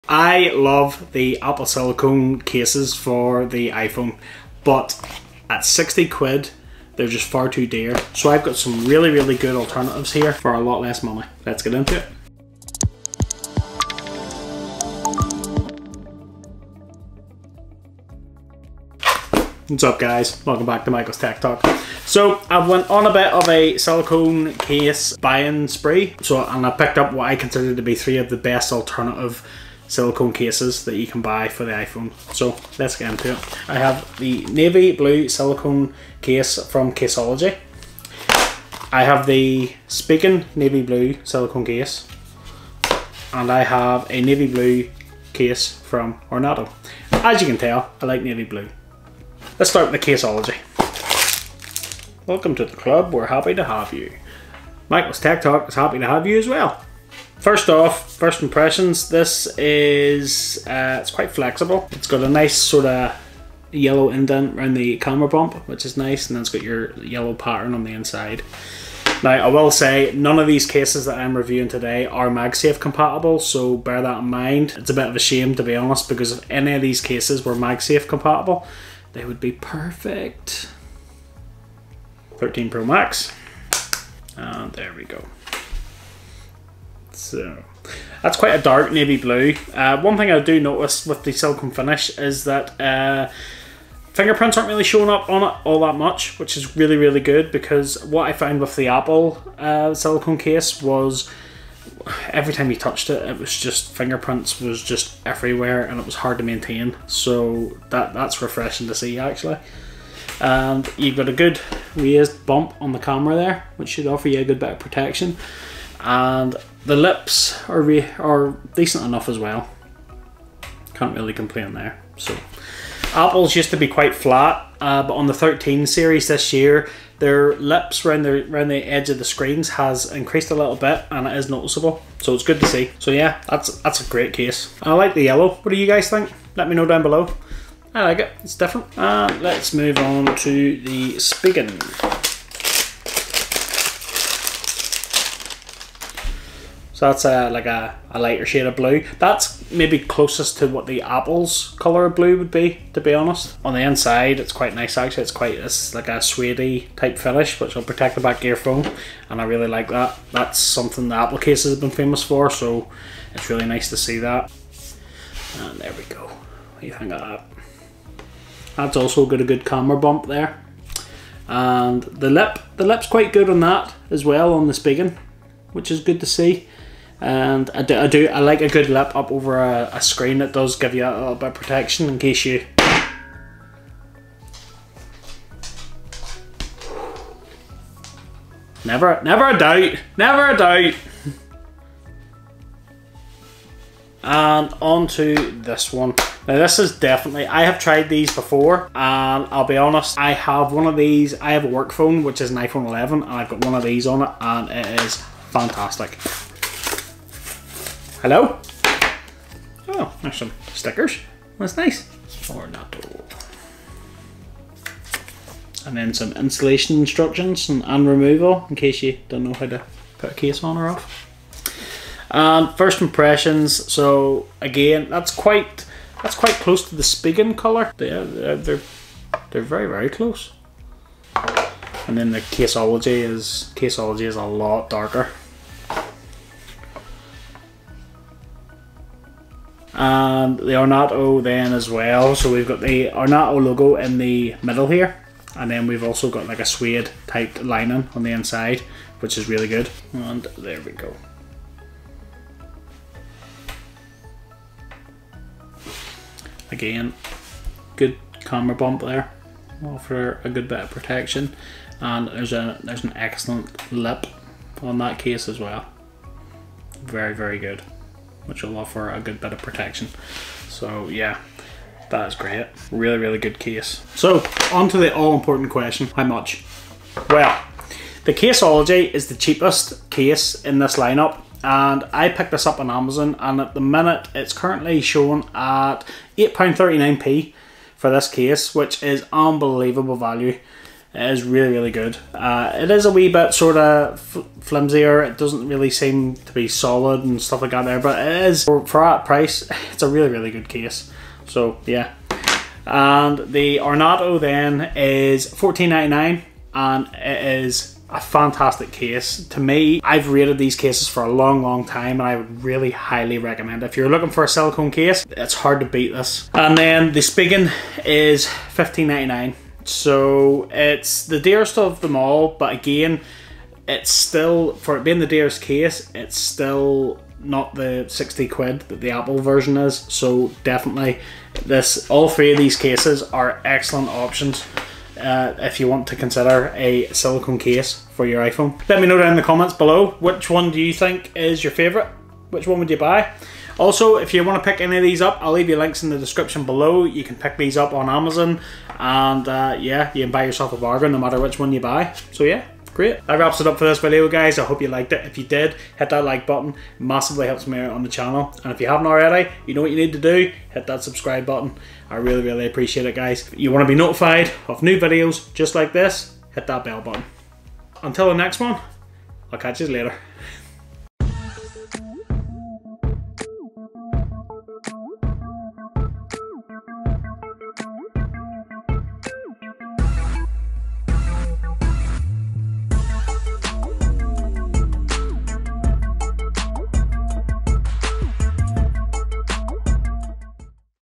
Silicone Cases for iPhone 13 Pro Max
Here i have some of the cheaper options for a silicone case for the iPhone 13 Pro Max, much cheaper and better quality than the Apple version that costs 3 times the price!

Caseology Nano Pop Silicone Case
Spigen Silicone Fit [Anti-Dust Technology] Case
ORNARTO Shockproof Liquid Silicone Case

0:00 - Intro
0:50 - Caseology Silicone Case
2:20 - Spigen Silicone Case
3:14 - Ornarto Silicone Case
4:38 - Prices
5:31 - Conslusion
5:56 - Outro